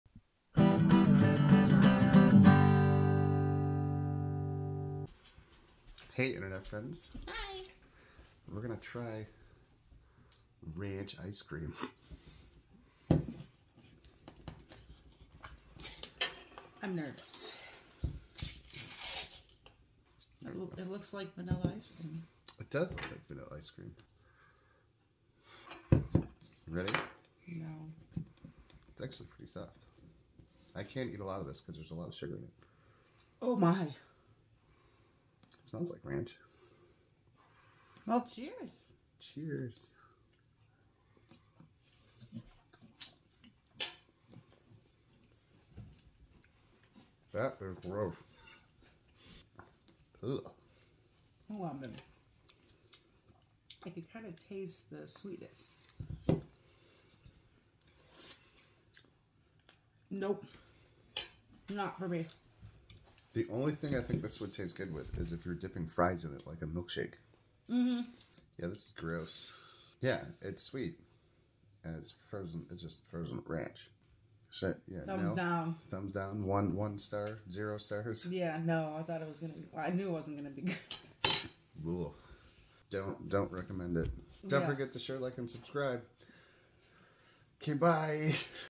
Tim Tries Ranch Ice Cream
Tim and Angie Try Ranch Ice cream